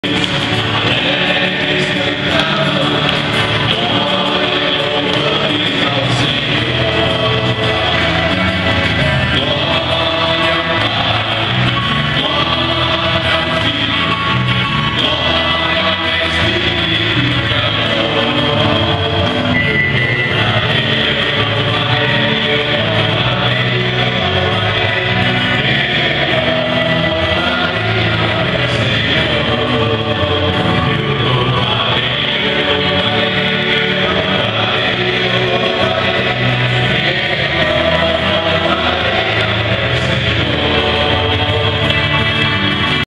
Missa Sertaneja 2013. Em 29 de junho, sábado, dia de São Pedro e São Paulo, tivemos um linda celebração, com a igreja ambientada com coisas "lá da roça".